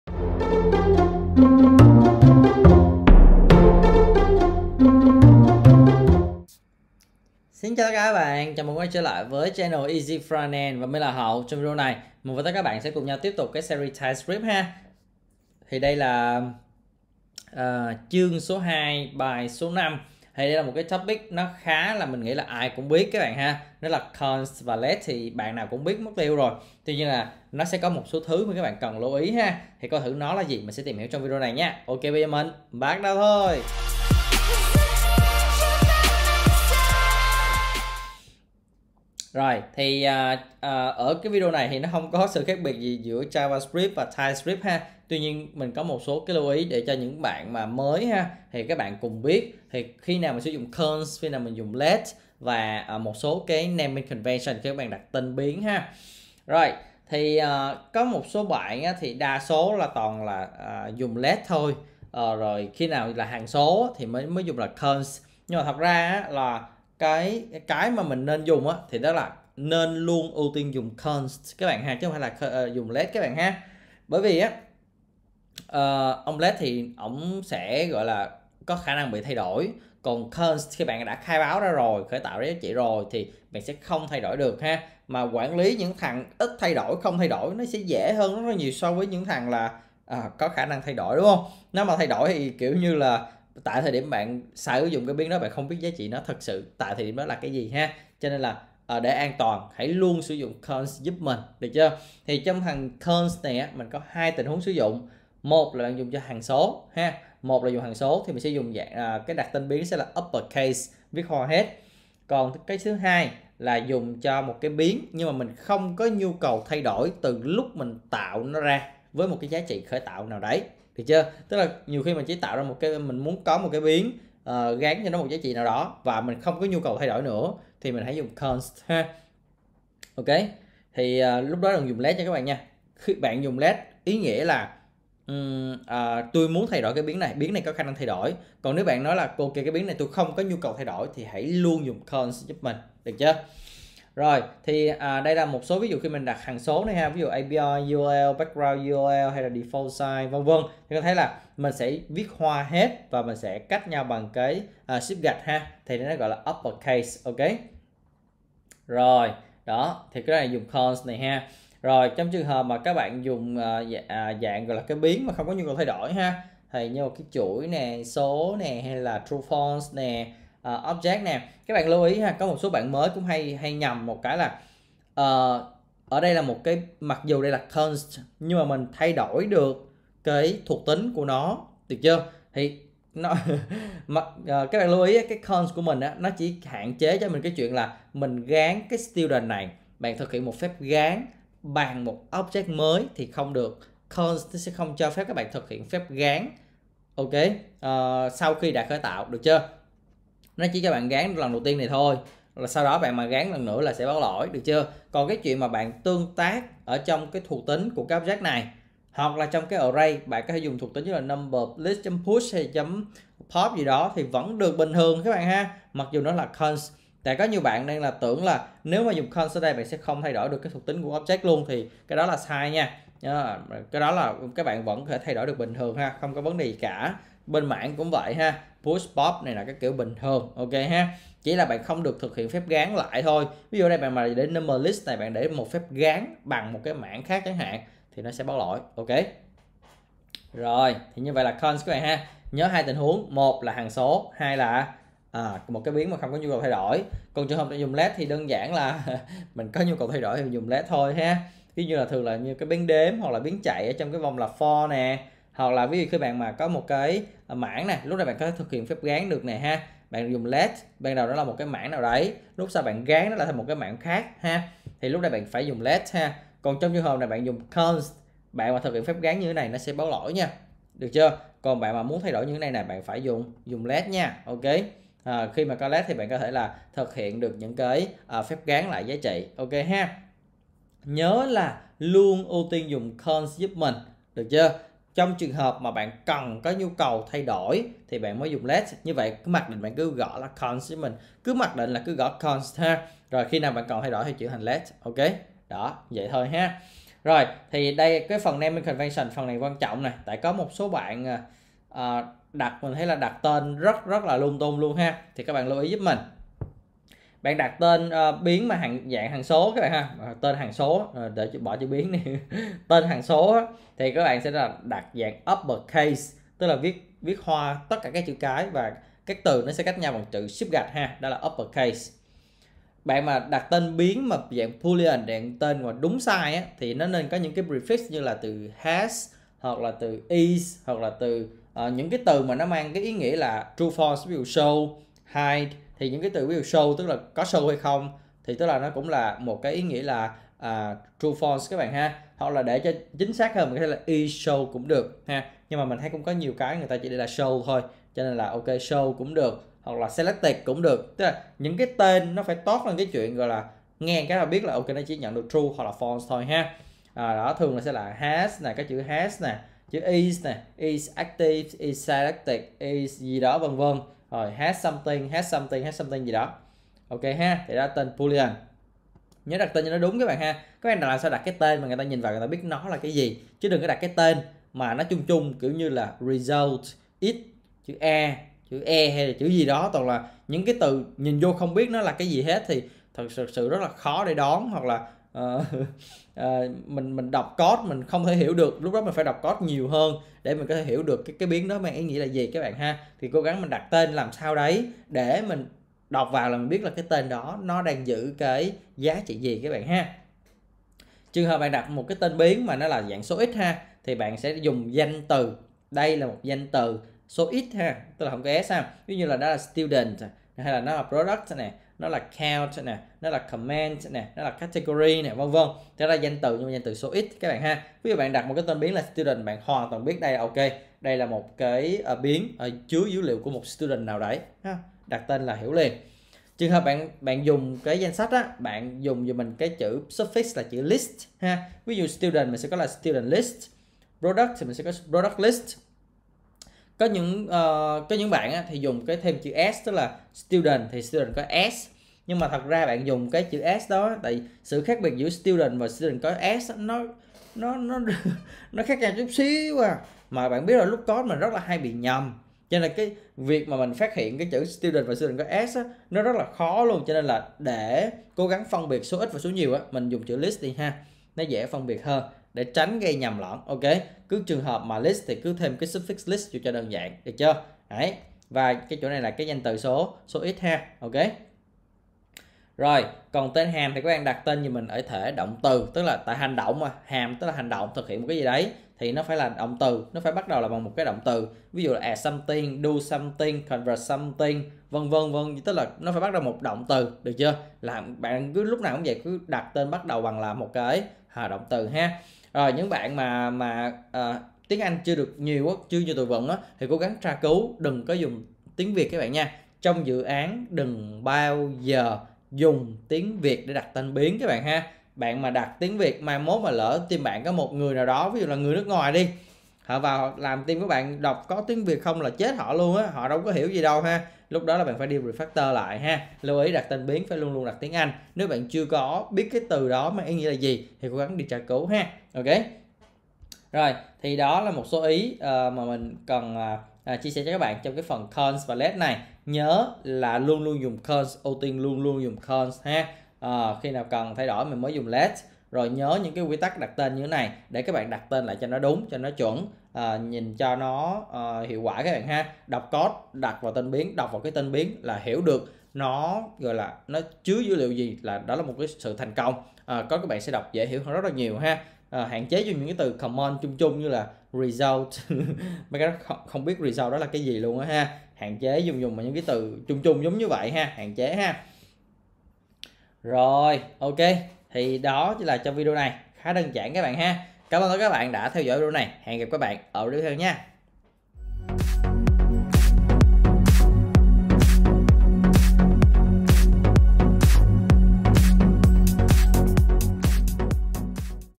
Typescript 02-05 const / let và cách đặt tên biến đúng chuẩn
Theo bạn thì nên ưu tiên dùng const hay let? Cùng mình phân tích trong video này nhé.

Video này không hẵn là nói về typescript, mà nó cũng giống như bên javascript, tuy nhiên nó khá quan trọng nên mình xin chia sẻ ở đây luôn hihi.

- const: hằng số + biến ko có nhu cầu thay đổi
- let: biến số mà sẽ thay đổi
- cách đặt tên biến bạn xem trong video nhé

VIDEO CHAPTERS:
Made by: Hoang Pham
Cảm ơn bạn nhiều nhé! ❤️

0:00 Giới thiệu
0:57 When to use const
4:07 When to use let
4:56 Naming convention
Easy Frontend - Code xịn hơn mỗi ngày
💰  Ủng hộ mình làm videos thì đóng góp qua MoMo/ZaloPay: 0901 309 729 nhé